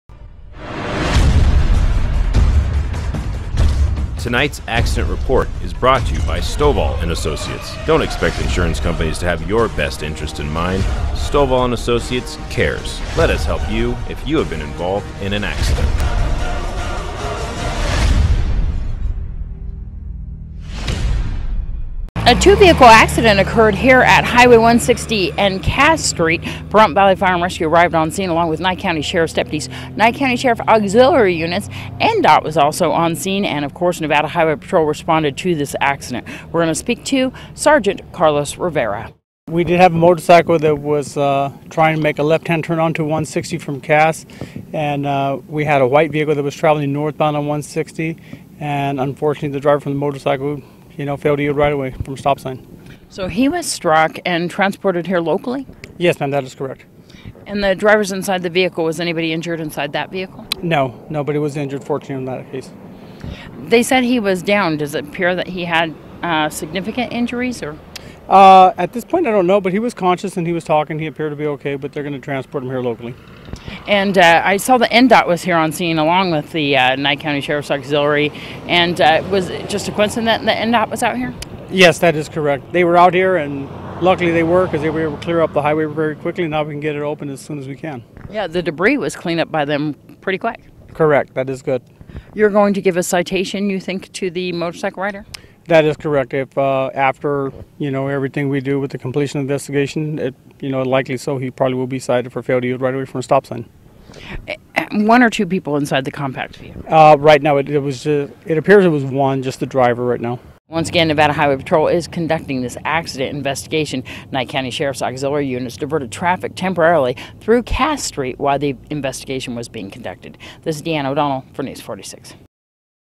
01/22/2013 Accident Highway 160 and CAAS
A rider of a motorcycle was transported this afternoon after colliding with a compact car on Highway 160 and CAAS.